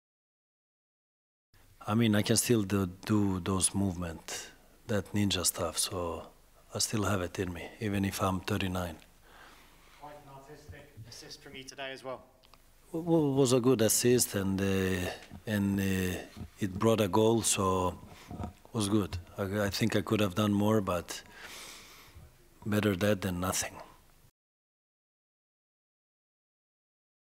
Ibrahimovic - "I can still do that ninja stuff" as he returns for Sweden
Zlatan Ibrahimovic started for Sweden for the first time in five years on Thursday, coming out of international retirement to set up the only goal in a UEFA World Cup qualifier against Georgia.

The 39-year-old AC Milan striker, who had not featured for his national side since Euro 2016, partnered Alexander Isak in attack and provided the assist for Viktor Claesson after 35 minutes.

Ibrahimovic became the oldest player to represent Sweden, beating the record of 38 years and 59 days previously held by goalkeeper Thomas Ravelli.

Sweden's greatest ever player has scored 62 goals in 116 caps.